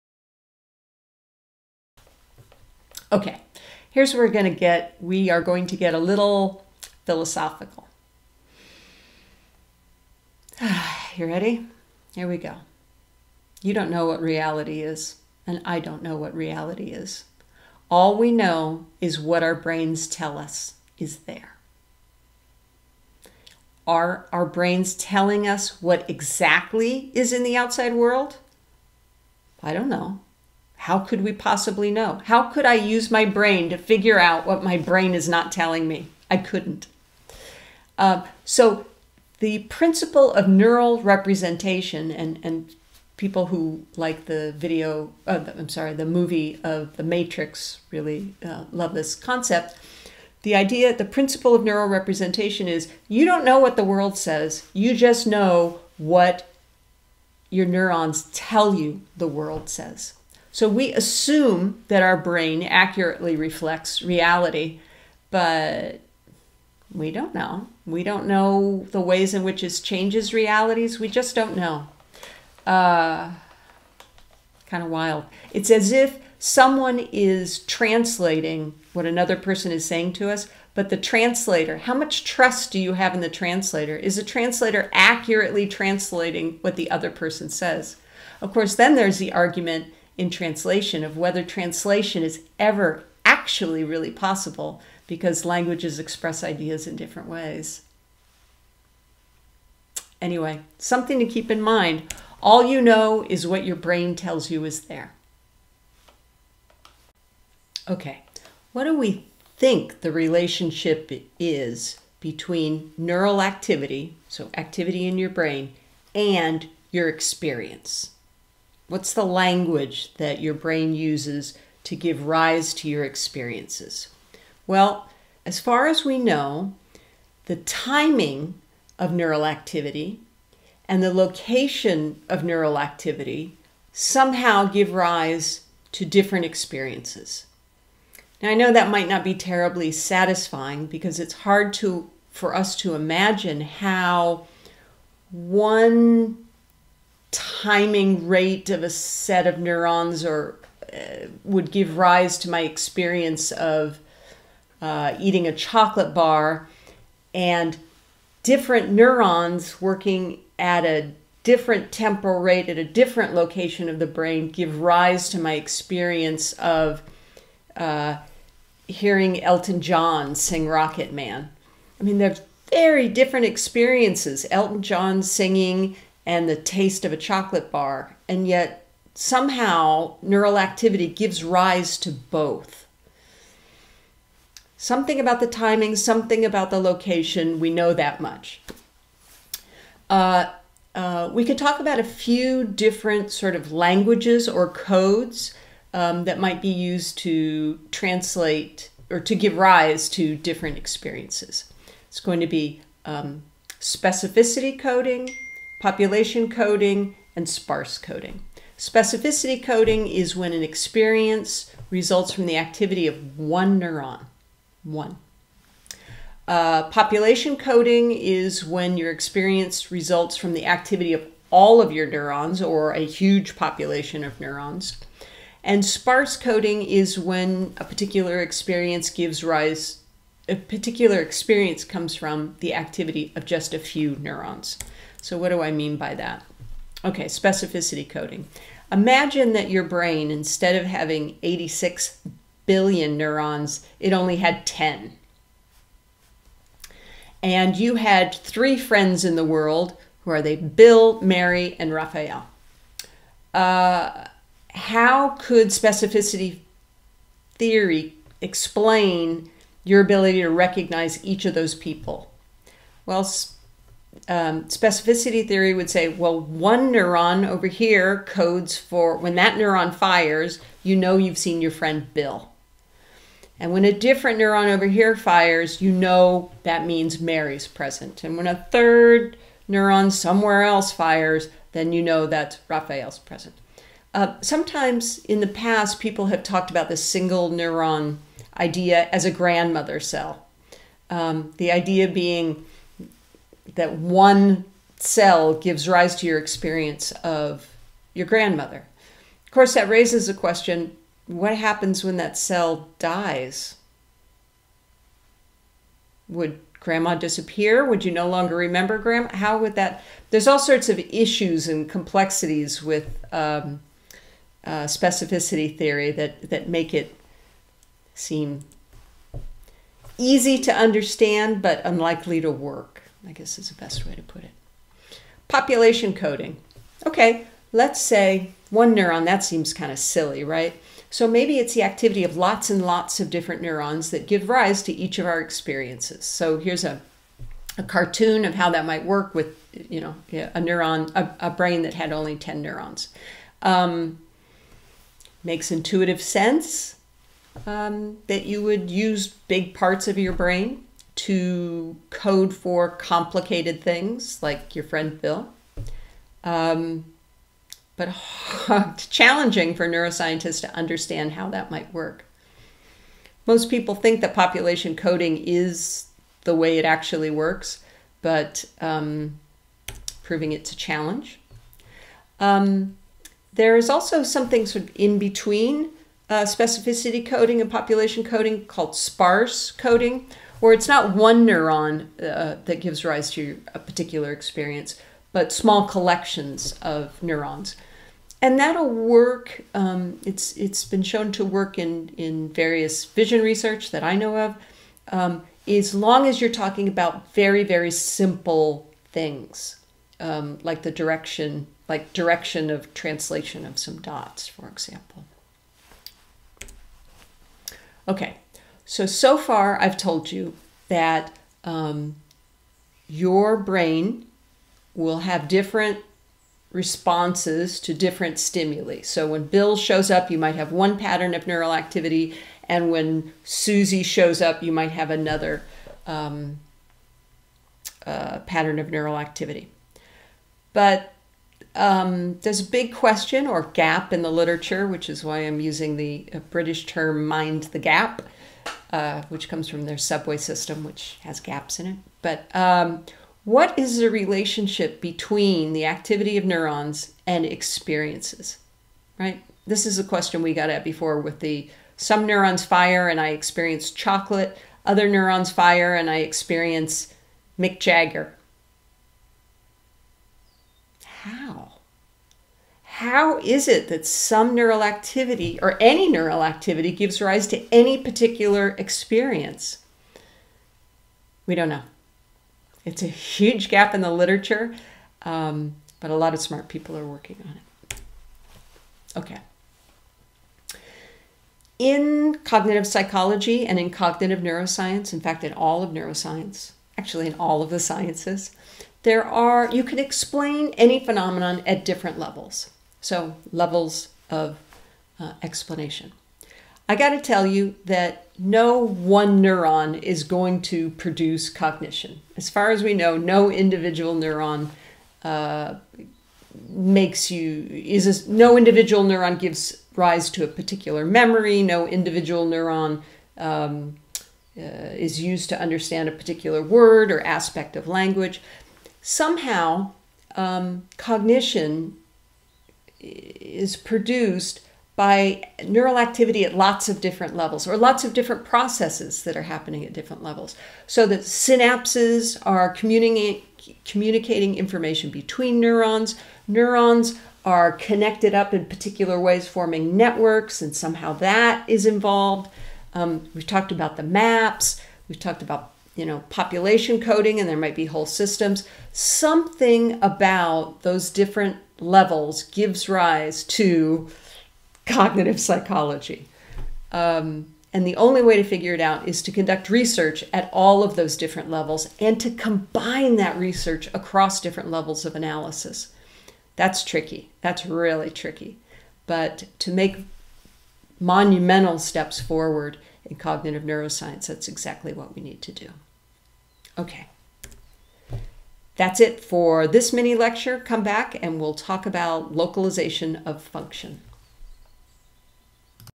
367 Lecture 4.4 Is Your Brain Telling You the Truth?
A discussion of the issues related to neural representation.  How does neural activity produce our various experiences?